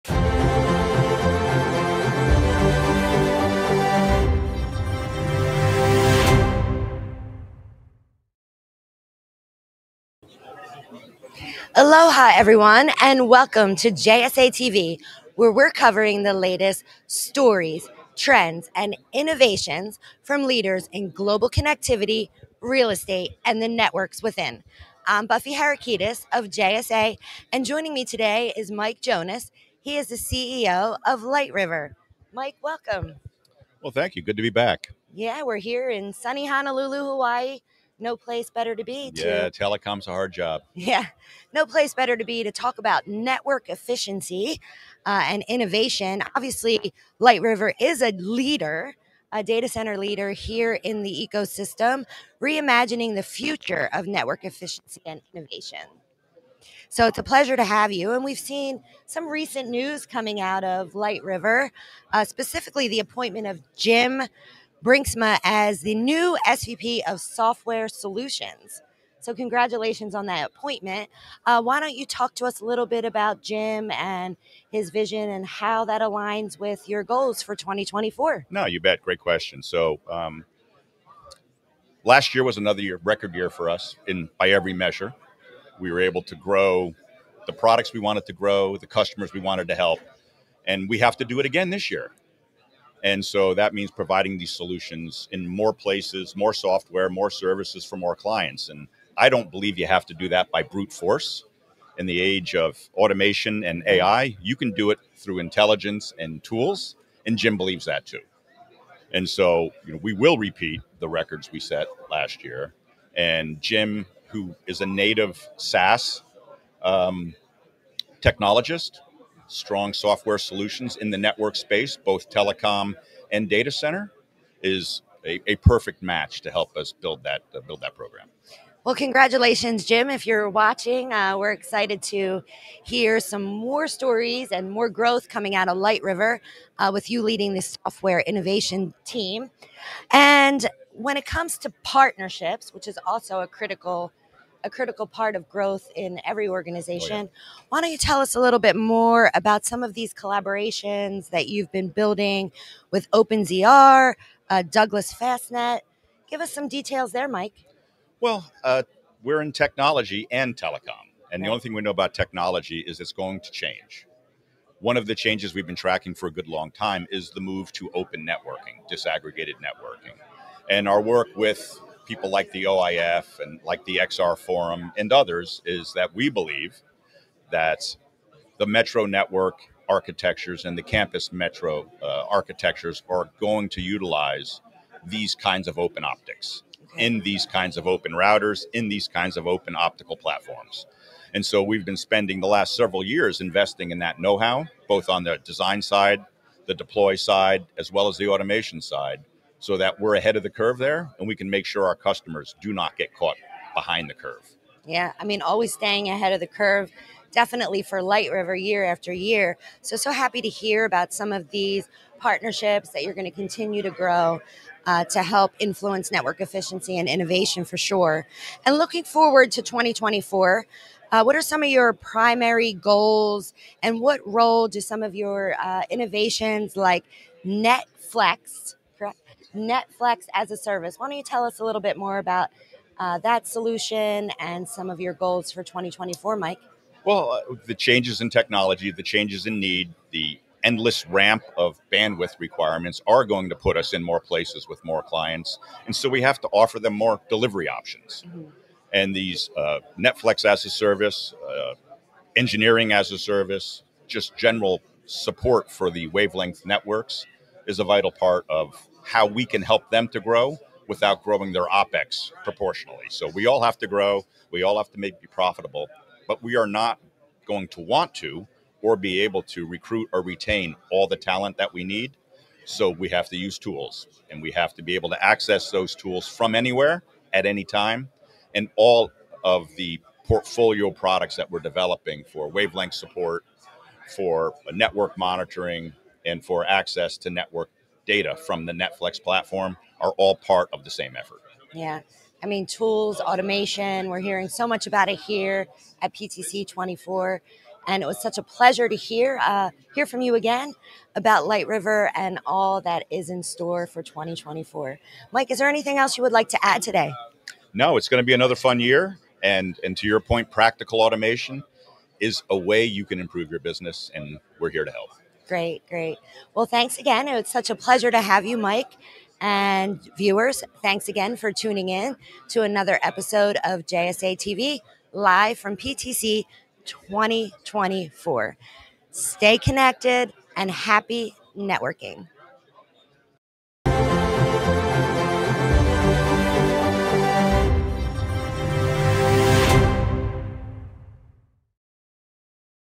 PTC’24 | LightRiver’s Mike Jonas: Pioneering Network Technology & Strategic Partnerships
Live from PTC, Mike Jonas, President of Global Customer Operations for LightRiver, discusses the company's latest growth, strategic developments, and partnerships. Discover how LightRiver is driving innovation and efficiency in 2024 and beyond.